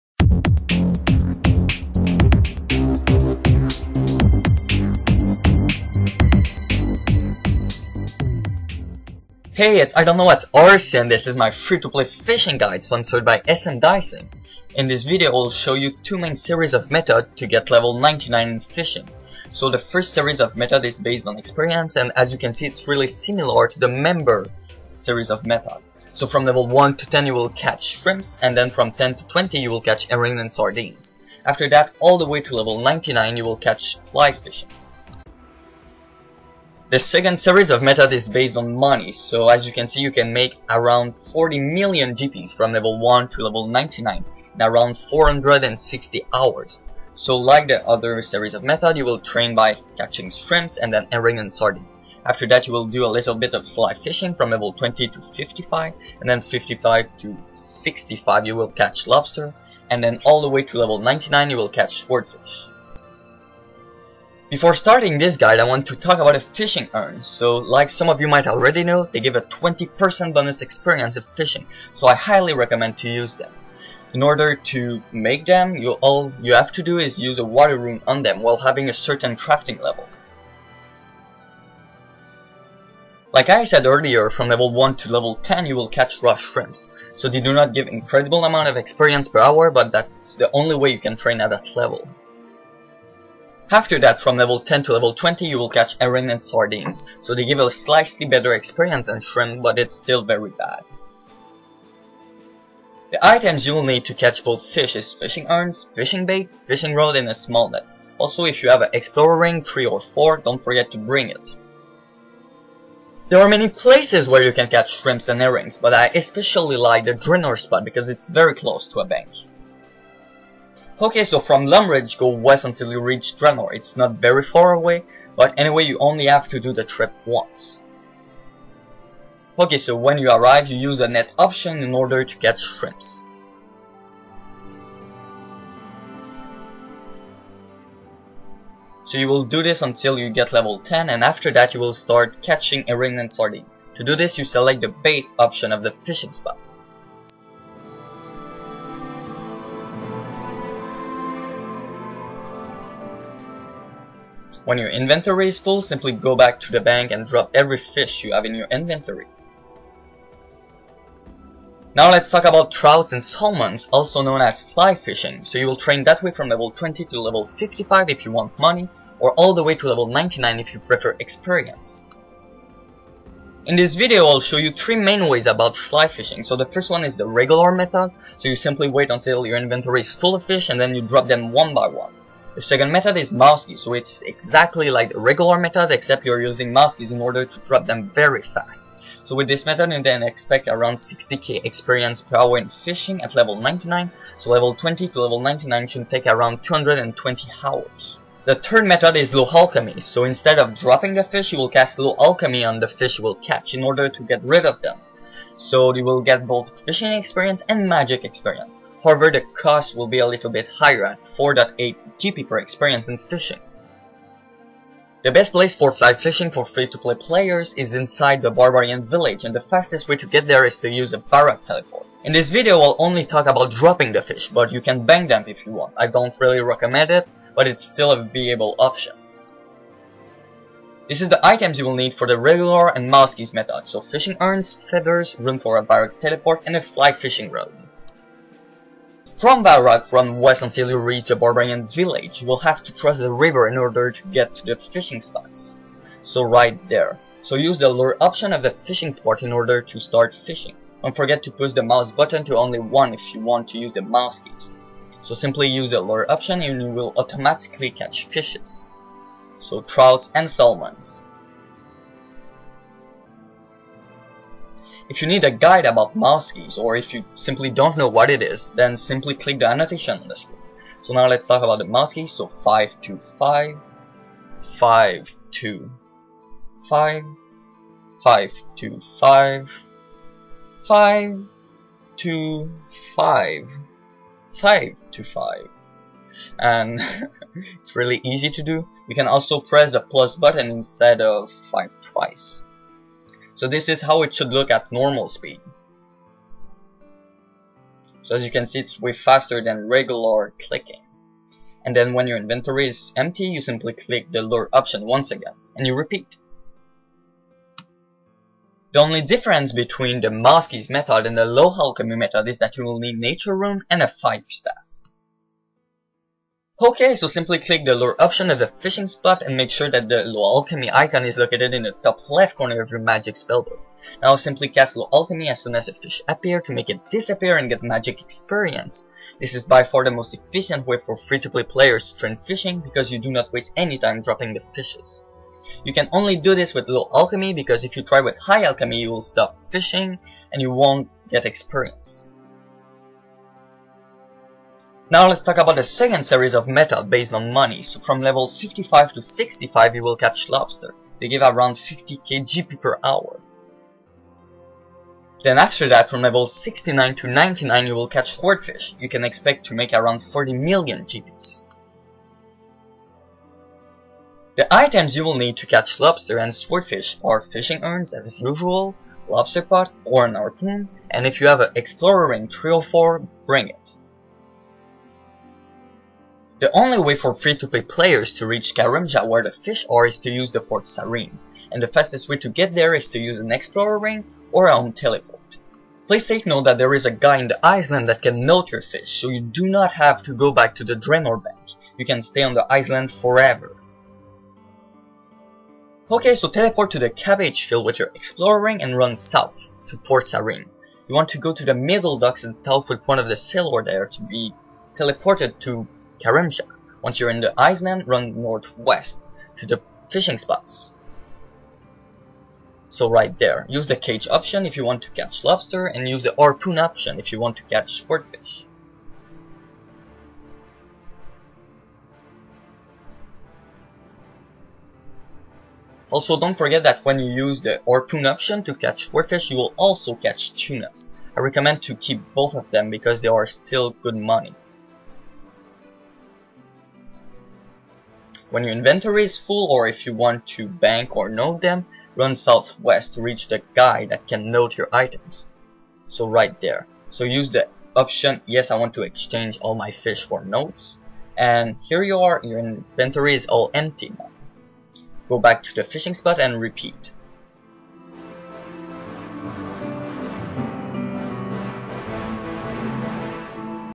F2P 1-99 Fishing Guide -Fly Fish (Low Alch & Mousekeys) Or Make 40m!- by Idk Whats Rc
Amount of Likes: 11,724 Next Reward: Let's Play (12,000)

This video shows two different series of methods to get level 99 Fishing. It includes a full guide on Fly Fishing, showing 3 main methods: Regular, Mousekeys and Low Alch.

Exp Influence 1:03
--- EXP--- 0:08
Level 1-10  Shrimp 1:21
Level 10-20  Herring/Sardine Fish 1:32
Level 20-99 Fly Fishing 2:40
TOTAL: 220h - 6m

---MONEY--- 0:35
Level 1-10  Shrimp 1:21
Level 10-20  Herring/Sardine 1:32
Level 20-99 Fly Fishing 2:40
Level 55-65  Lobster 5:43
Level 65-99  Swordfish 5:54
Total: 460h + 43.5m